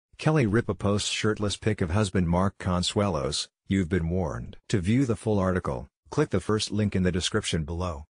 Kelly Ripa posts shirtless pic of husband Mark Consuelos: Youve been warned
Kelly Ripa is a proud wife. The morning show personality, 49, posted a photo on social media of her bare-chested husband, Mark Consuelos, getting a sweat at a gym with his "Kingdom" co-starsJonathan TuckerandMac Brandt. "Warning: this show is brilliantly written, beautifully acted and jam packed with male nudity, youve been warned," she wrote. KELLY RIPA, MARK CONSUELOS SAY COMFORT AND EXPERIMENTATION IS KEY TO THEIR HEALTHY SEX LIFEThedrama series dropped on Netflix on July 1 and is abouta retired MMA fighter.  It originally ranfrom 2014 to 2017 on Direct TV. Consuelos previously told Entertainment Weeklythat working with "Kingdom" showrunner Byron Balasco was a great experience. Its not often that you get someone that writes characters like this, even secondary and tertiary characters off to the side," he said.  "Theyre really full and interesting. "Consuelos only problem was his age.  "Guys still think were 18 years old at all times, which is hilarious," he joked. KELLY RIPA REVEALS SHES SECRETLY BEEN TAPING LIVE FROM THE CARIBBEANRipa and Consuelos returned to New York after spendingseveral weeksin the Caribbean during the coronavirus pandemic lockdown. The couple and their three children, Michael, Lolaand Joaquin, were supposed to be on a short beach vacation but ended up sheltering in place whenflights were canceled because of the outbreak. Ripa began carrying out her daily "Live" hosting duties from the tropical destination. RyanSeacrest and I both decided it was better to do the show because we just felt like with everything going on in the world that perhaps, some normalcy just doing what we normally do, even though it was like in very unusual circumstances, would be the best thing for us," she explained. Fox News Melissa Roberto contributed to this report.